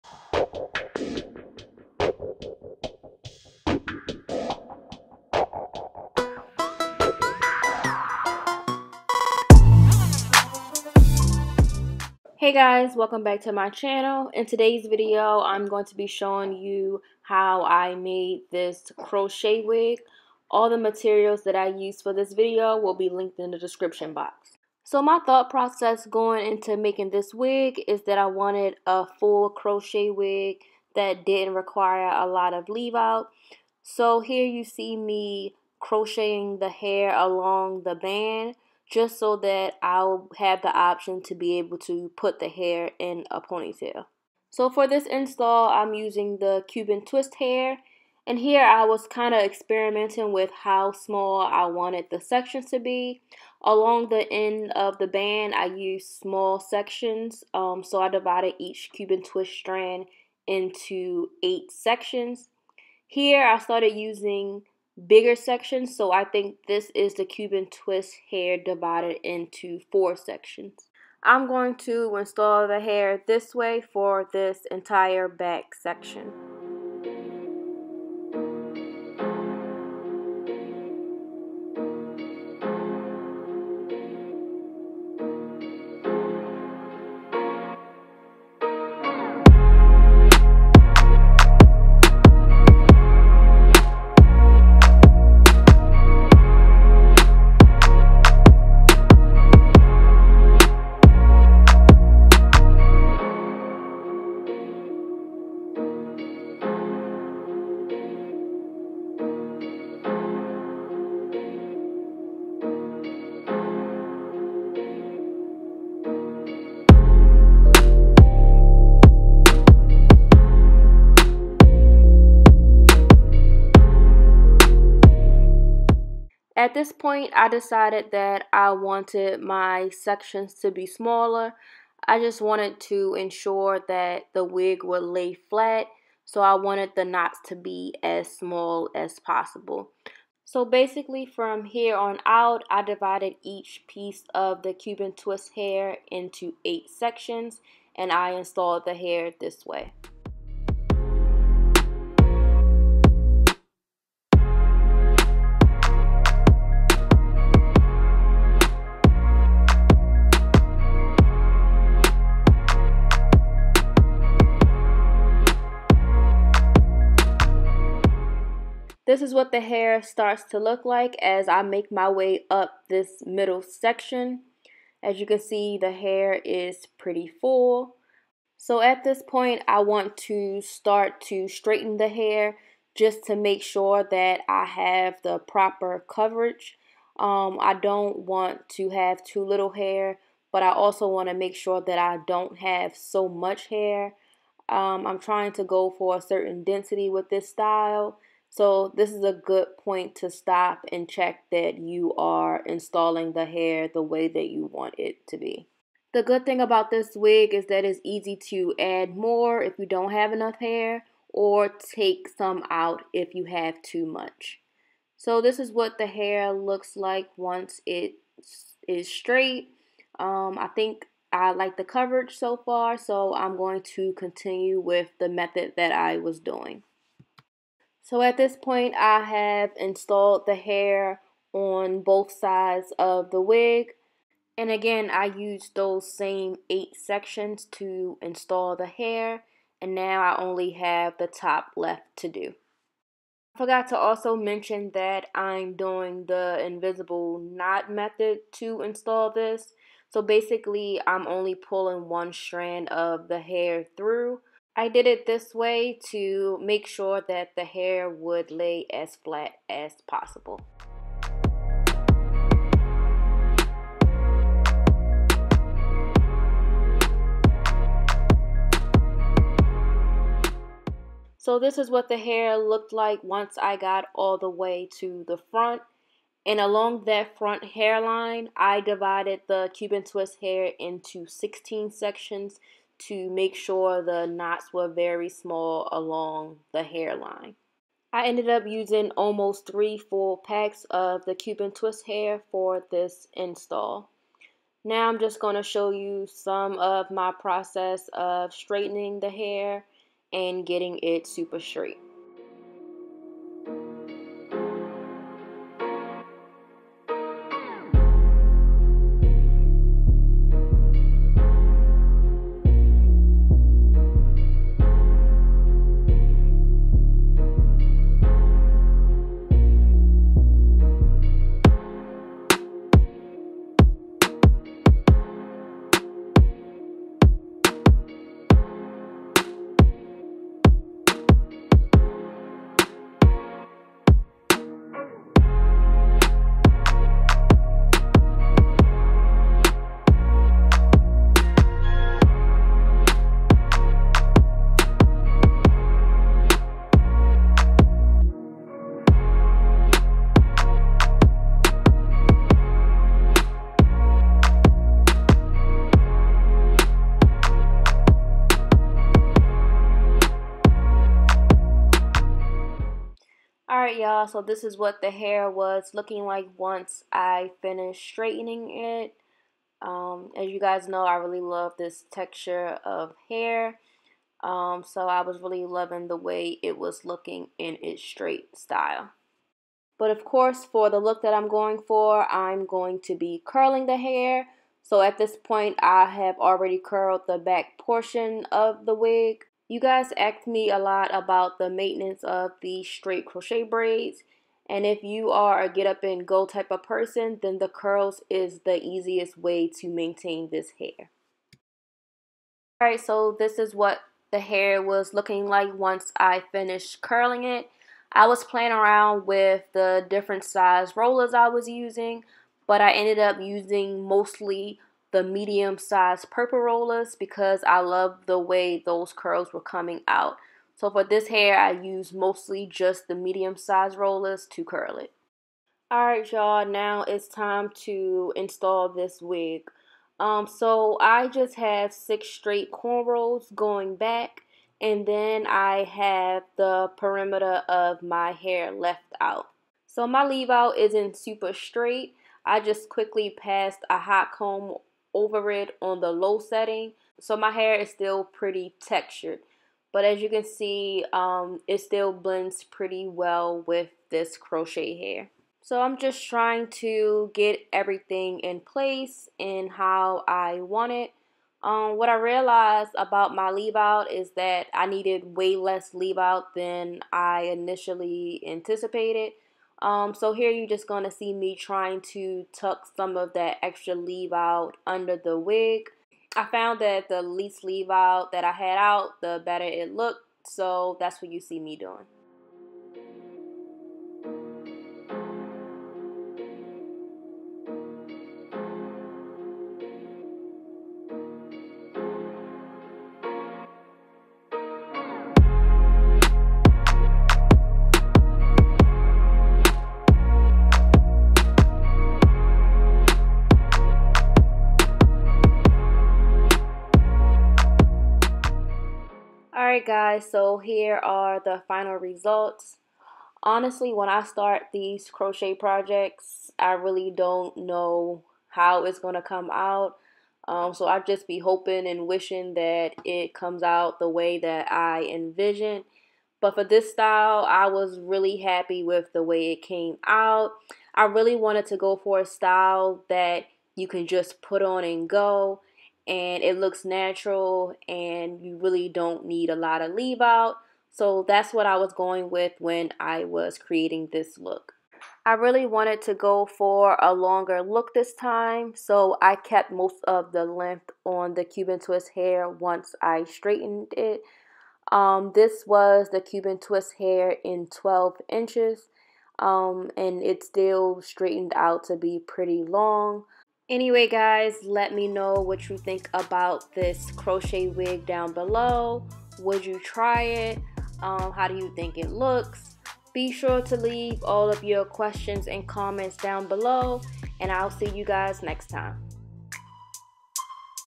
Straight Crochet Wig w/ Minimum Leave Out!
Hey Y'all! In today's video, I'll be showing you how I made this straight crochet wig using the cuban twist hair! This wig is a great protective style that doesn't require a lot of leave out. Let me know what y'all think!

Hair/Tools Used:
Freetress Equal Cuban Twist 12"
Crochet Wig Cap
One Step Hair Dryer Brush
Hot Rollers